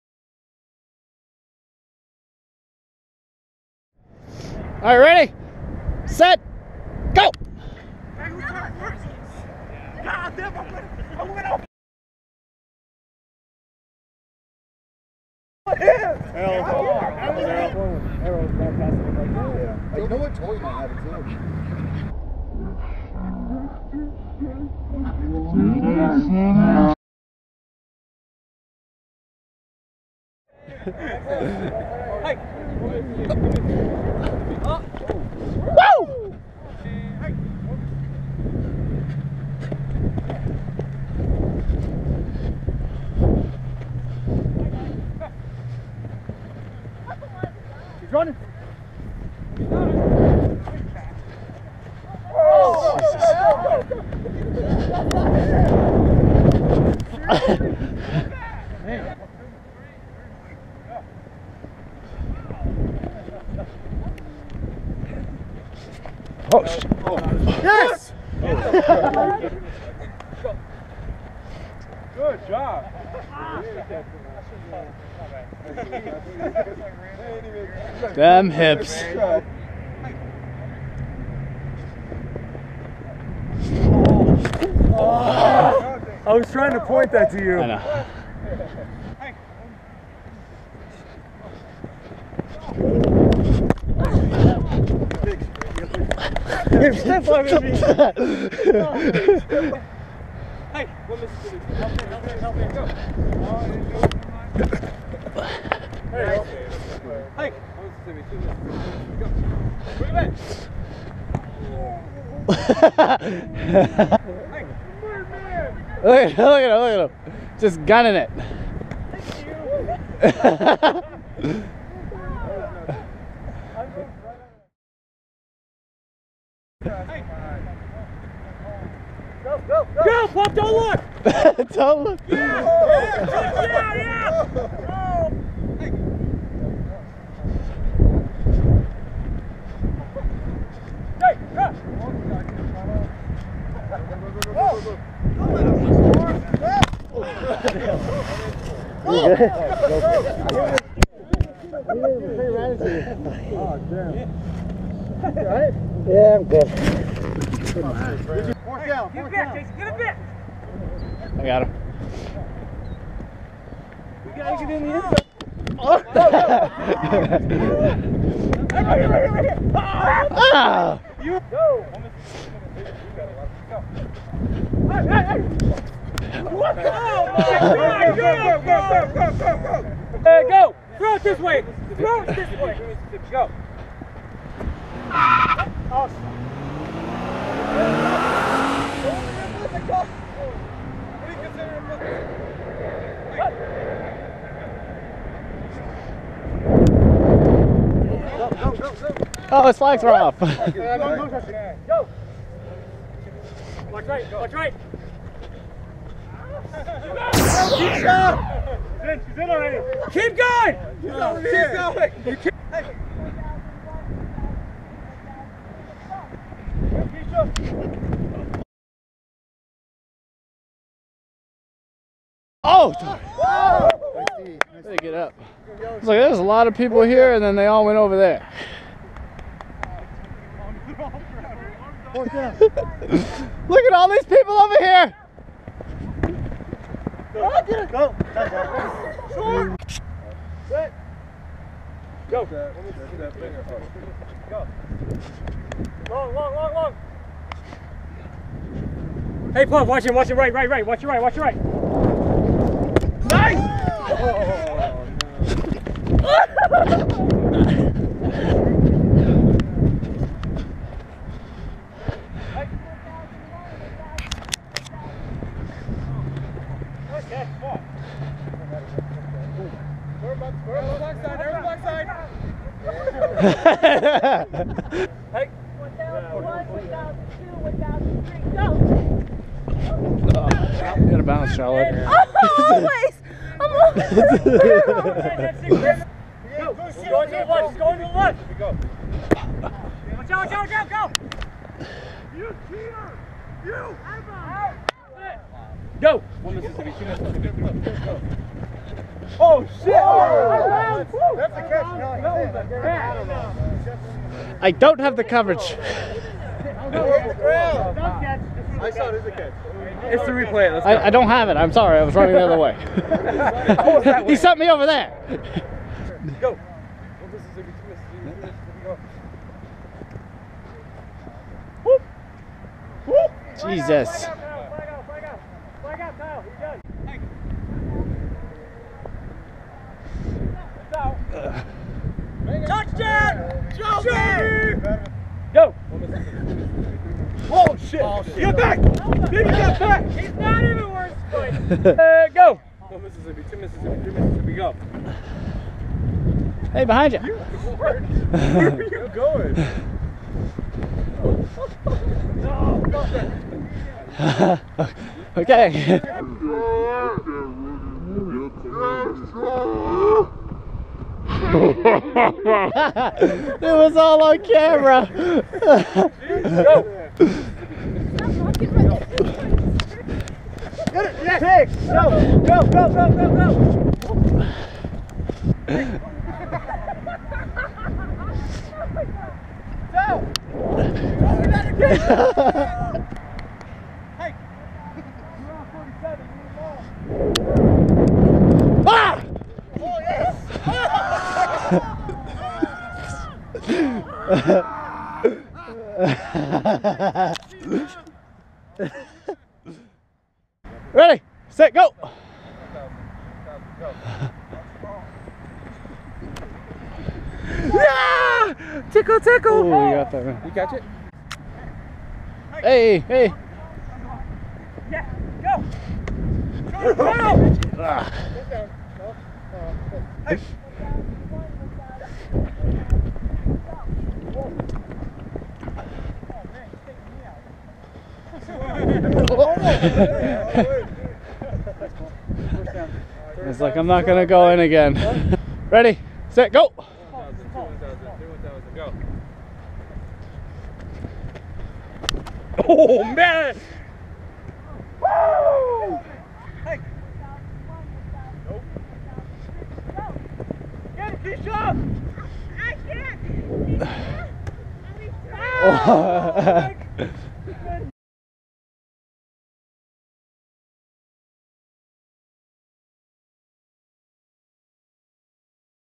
Tuesday Flag Football TOP Plays 3.3.2015 (Part II)
PART II of TOP Plays from Tuesday Flag Football at Sunset Park! Enjoy and thank you!